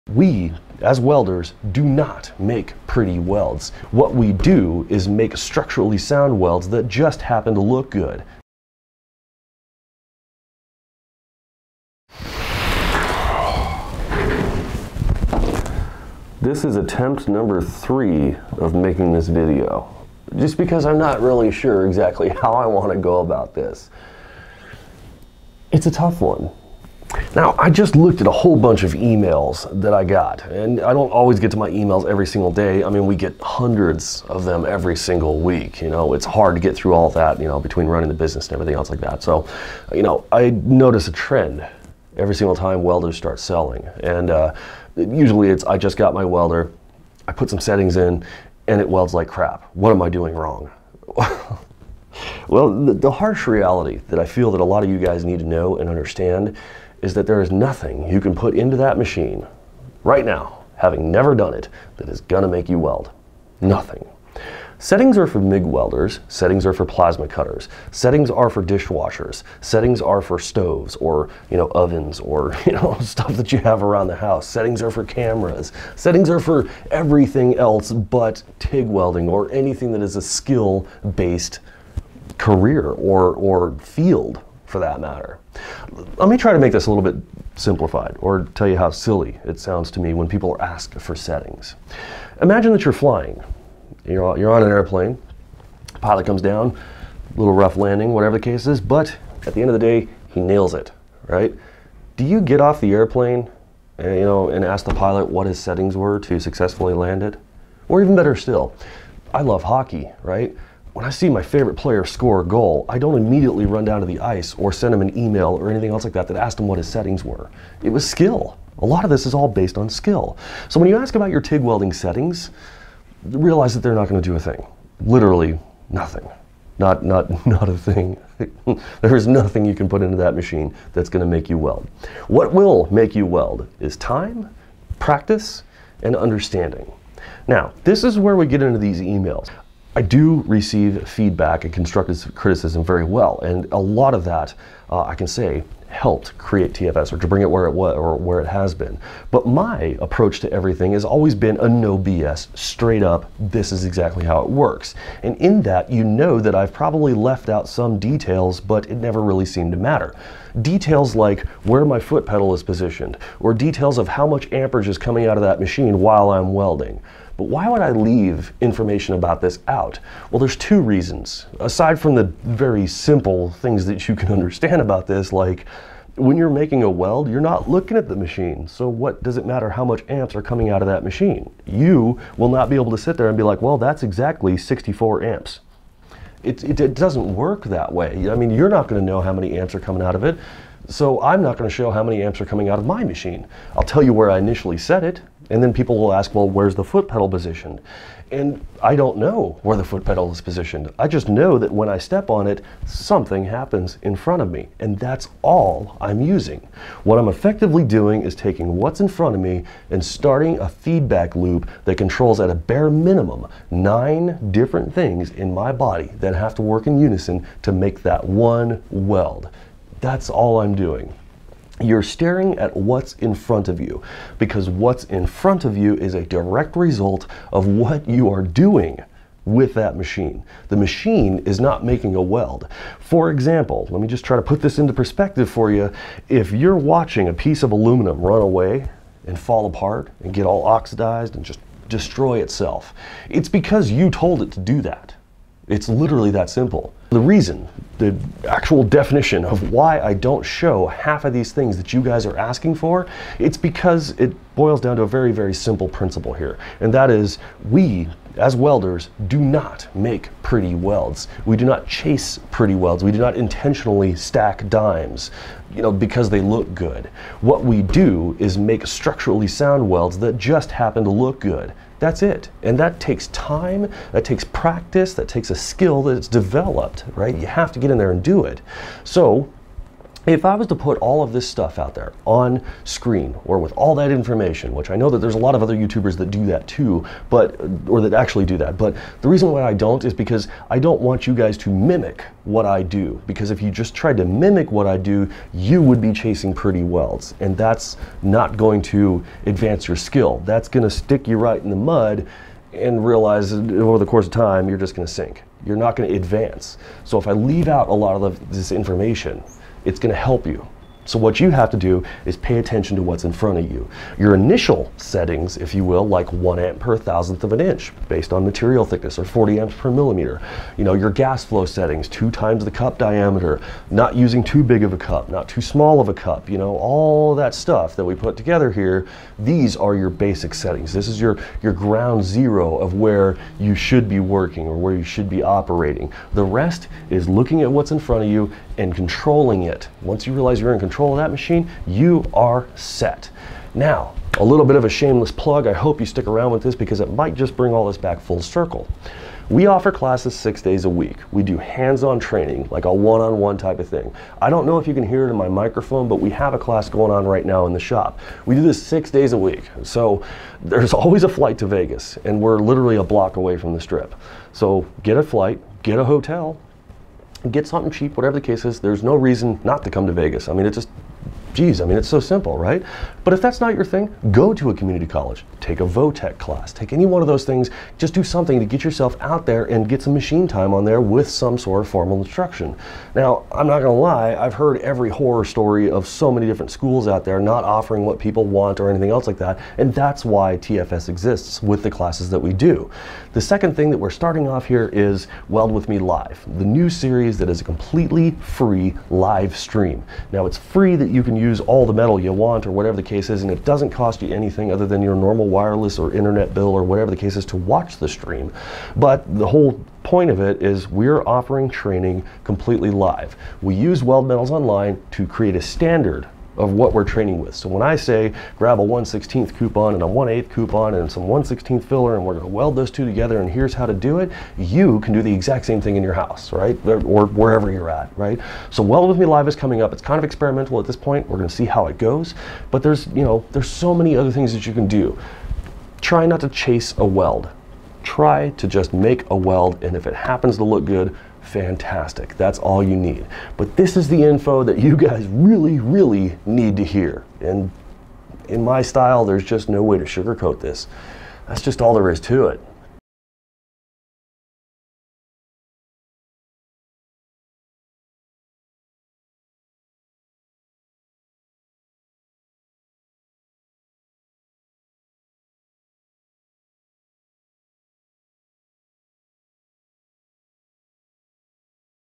TIG Settngs NOBODY is Talking About
The biggest kept secret of TIG settings finally revealed in the most straight up approach possible.

Learn to TIG Weld in Our Class!